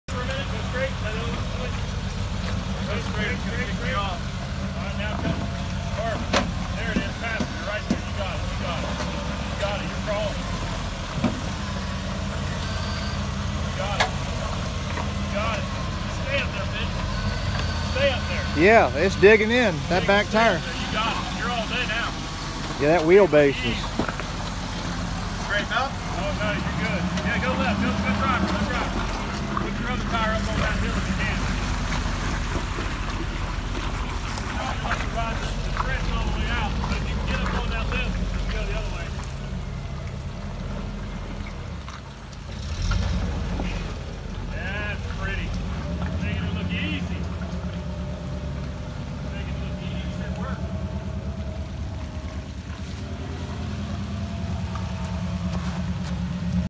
M715 at River Rock Orv Dec2018
Matanzas Jeep club ride at River Rock